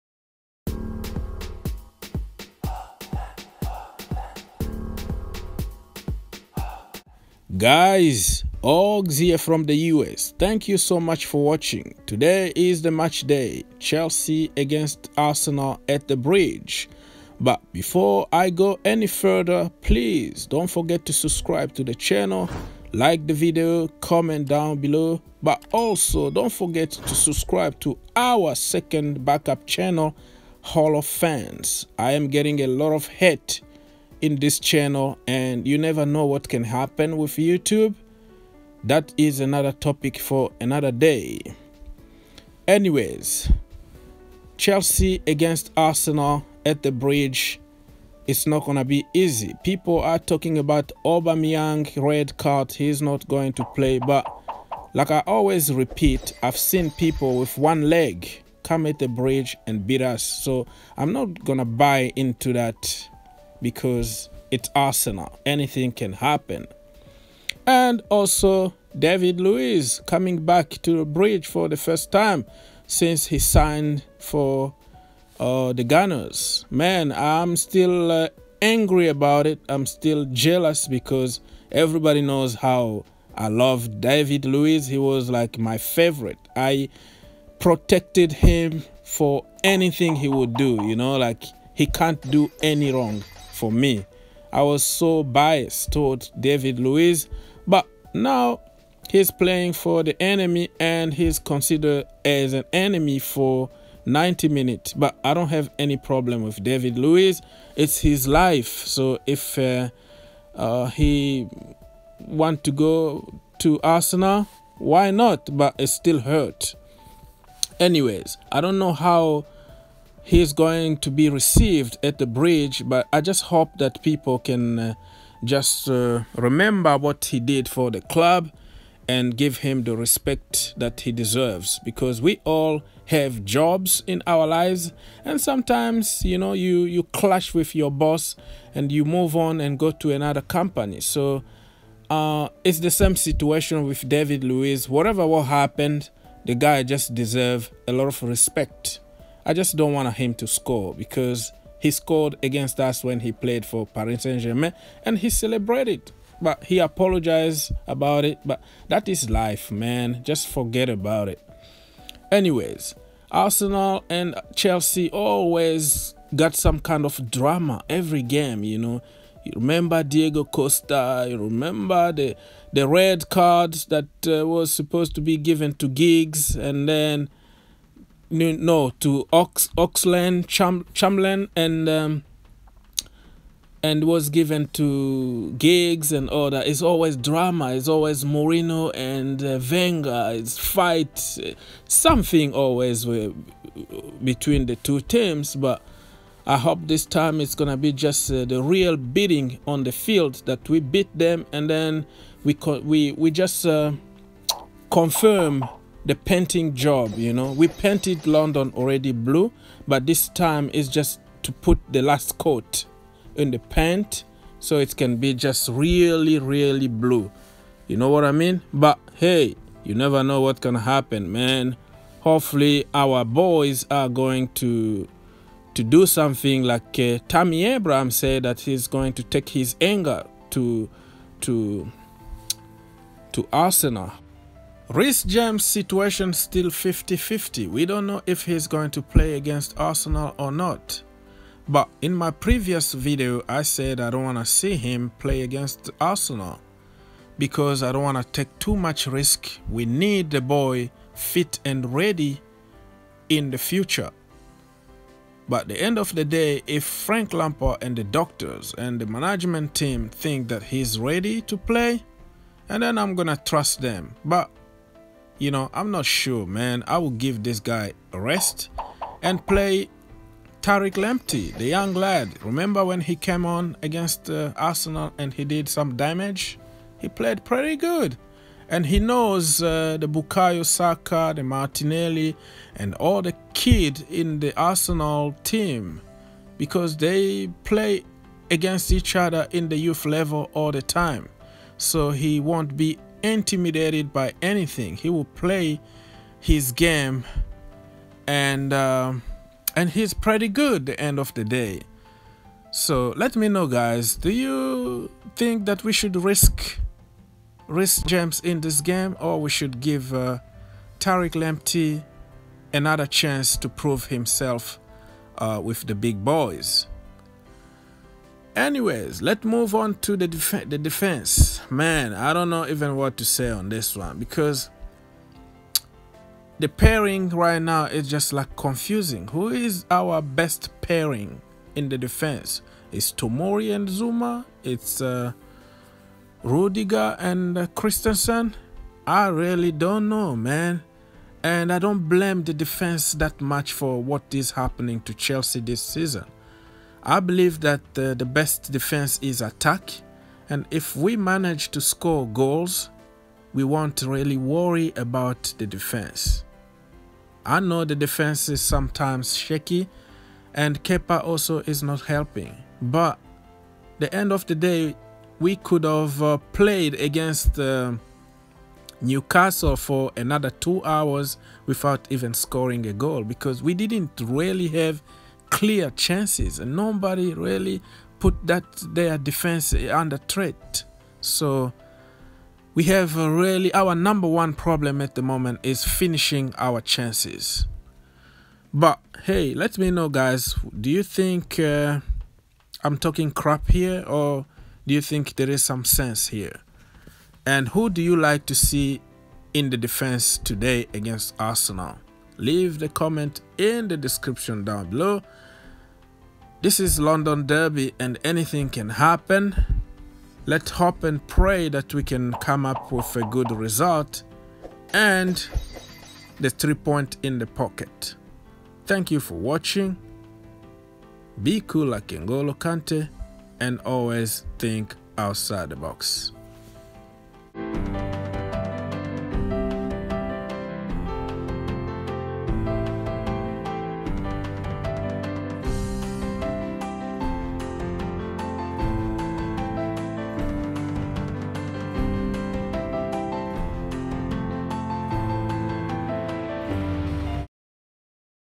DAVID LUIZ BACK AT THE BRIDGE | TARIQ LAMPTEY DESERVES A CHANCE (CHELSEA VS ARSENAL PREVIEW)
Chelsea vs Arsenal Preview
Frank Lampard full press conference